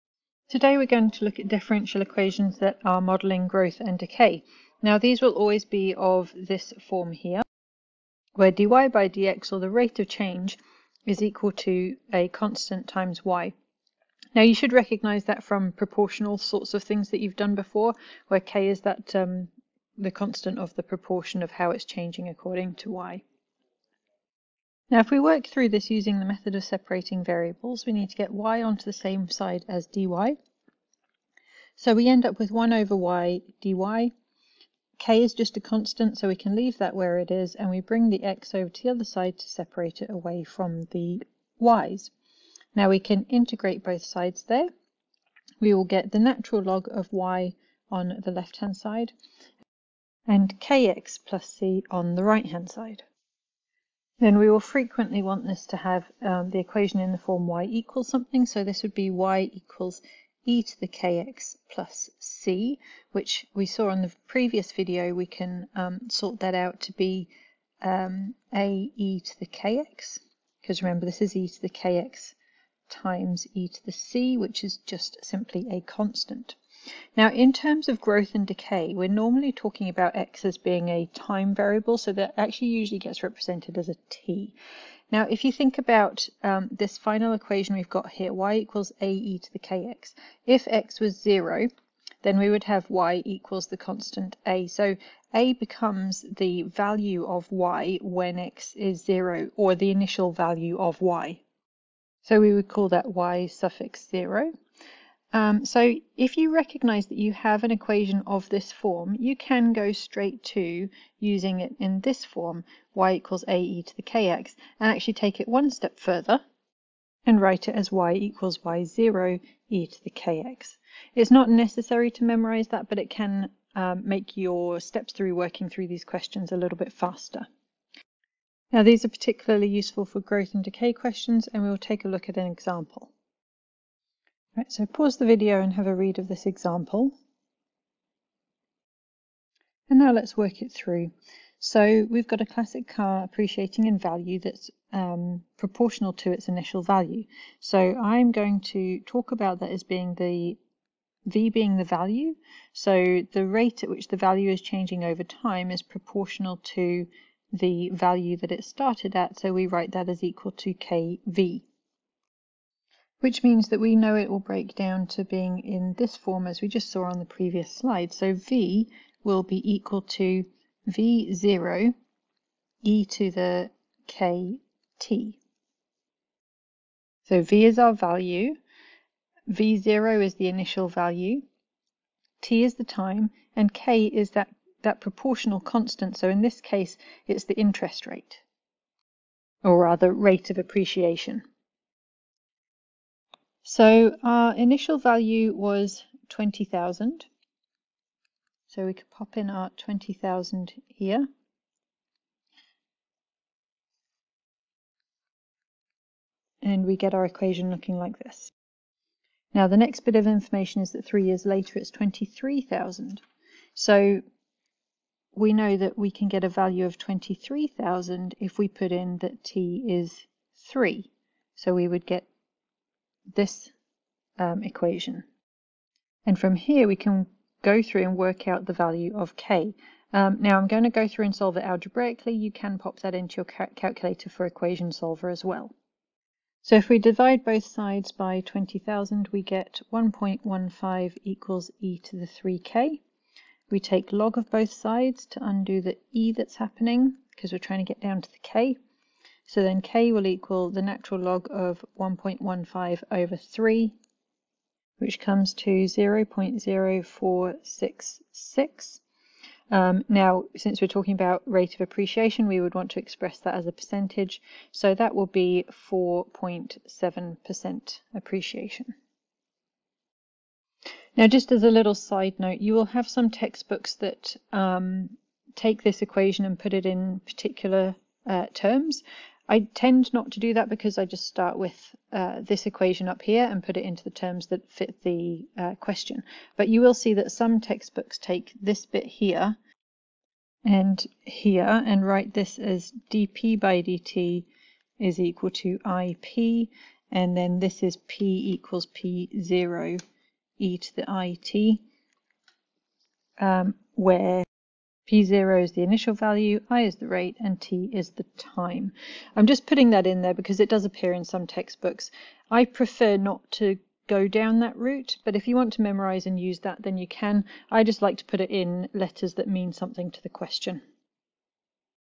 Differential Equations for Growth and Decay
Using differential equations to solve growth and decay problems